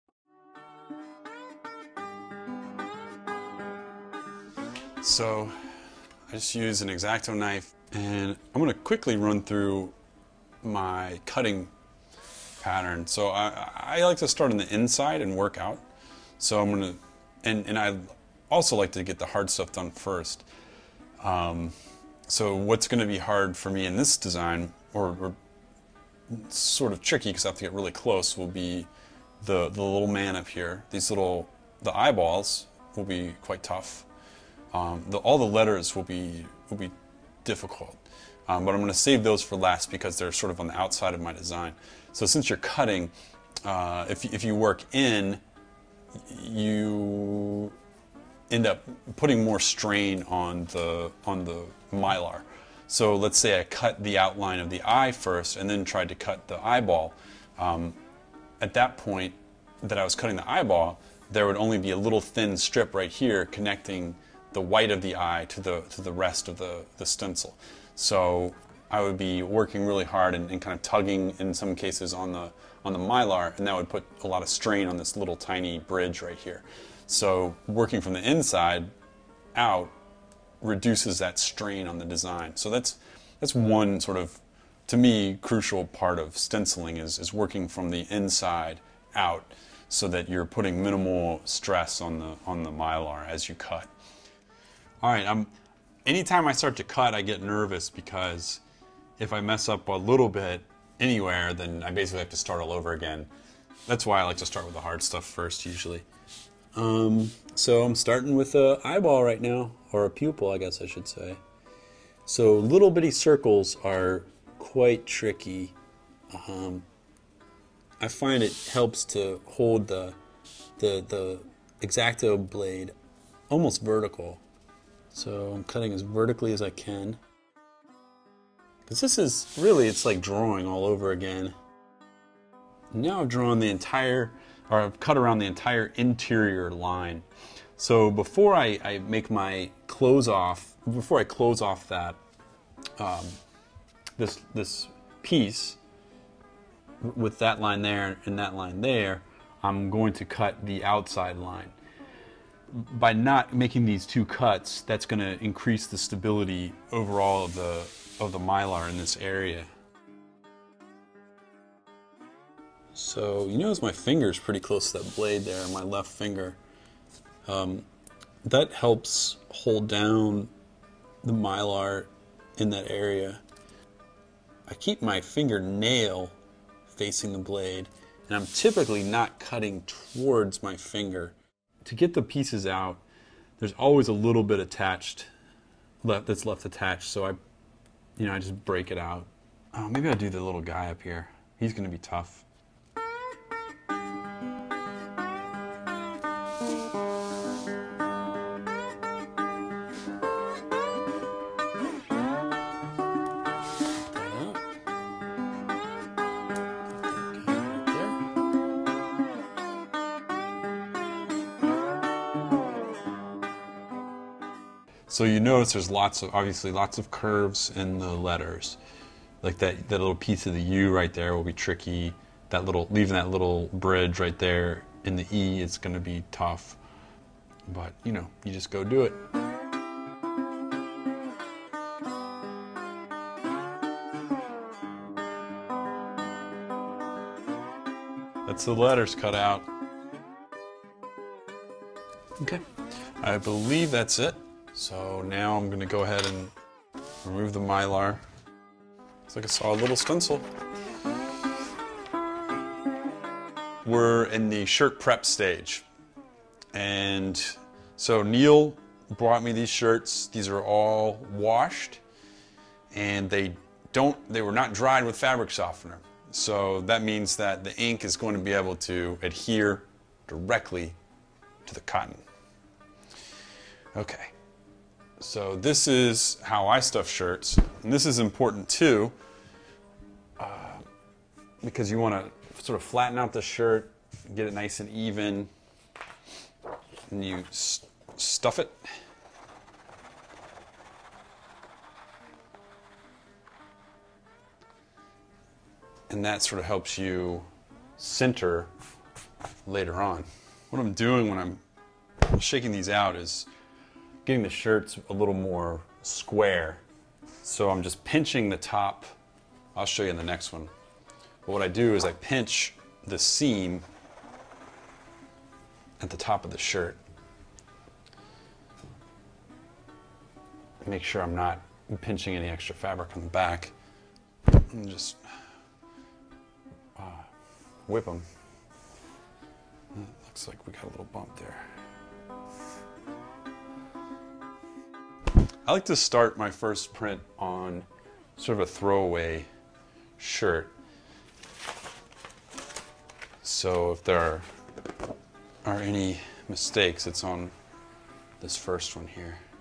How To Stencil Screen Print T Shirts (3-4)
3) Cutting Stencils
4) Prepping the Shirts

Instruction on a technique for stencil screen-printing t-shirts, from Garrettshirts.com